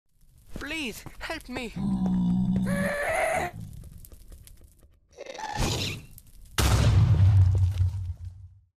Short-Animation | Nether
► Lire La Description | IMPORTANT |

• Logiciel Utilisé : Cinema 4d - Camtasia Studio 7 - AfterEffects

Je m'ennuyais et je sais pas pourquoi j'ai voulut vous faire une petite animation pour le fun :p
Je tient juste à préciser (au passage) que l'animation à beaucoup de bug de lumière mais ce n'est pas une animation "officiel" donc j'ai préférer ne pas m'attarder dessus ^^
On peut dire qu'elle servira de "mise en bouche" pour vous montrez une aperçu de ce que je peut faire en 1 journée :)